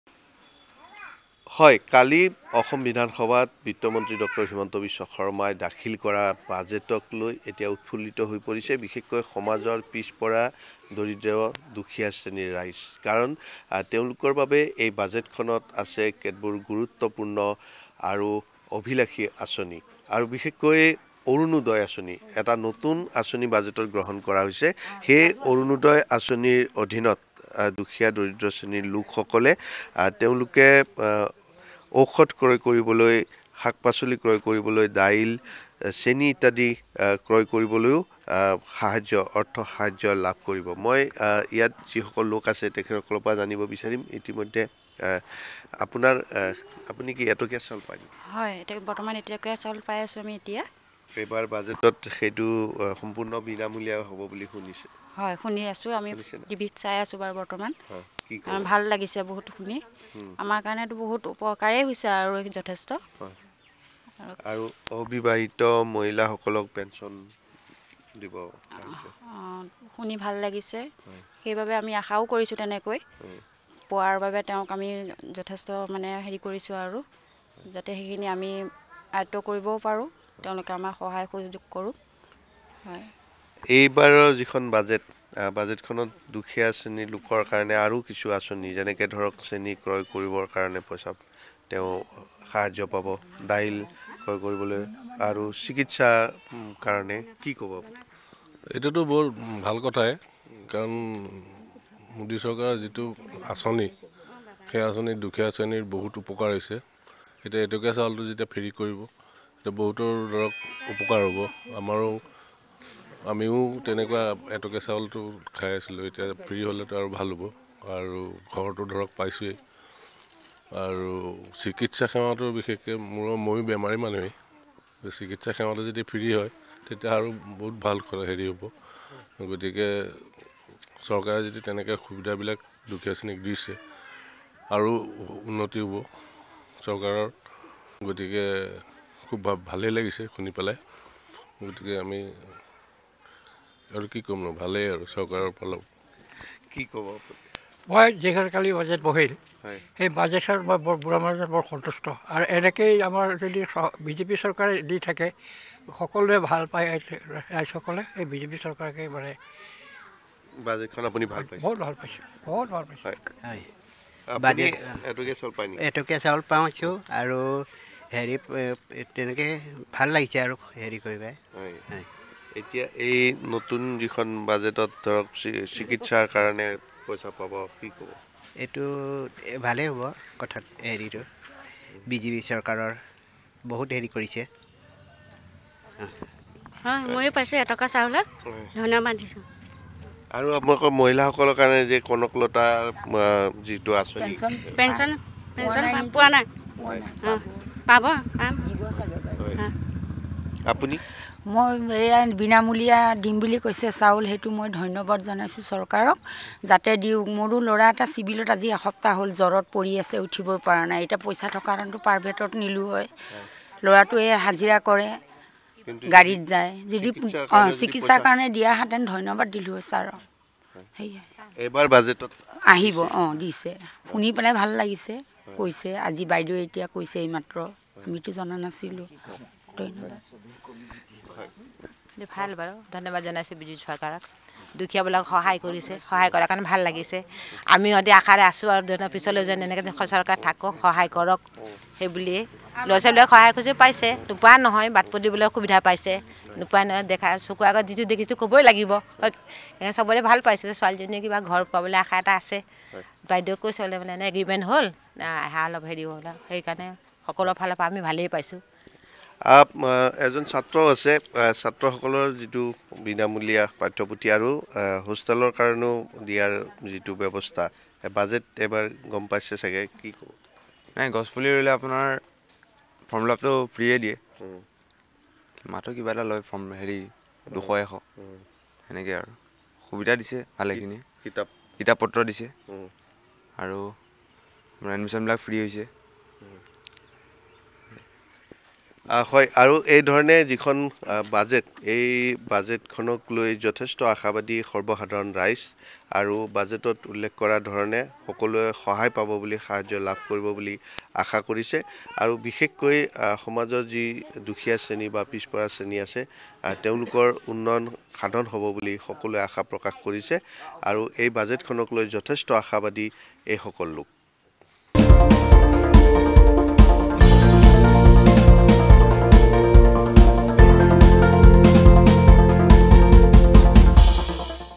বাজেট দাখিলৰ পিছতেই সকাহ অনুভৱ কৰিছে দৰিদ্ৰ পৰিয়ালসমূহে
ৰাজ্যৰ দৰিদ্ৰ আৰু অৰ্থনৈতিকভাৱে অনগ্ৰসৰ পৰিয়ালৰ বাবে বাজেটত বিশেষ ব্যৱস্থা। বাজেট দাখিলৰ পিছতেই সকাহ অনুভৱ কৰিছে দৰিদ্ৰ পৰিয়ালসমূহে ।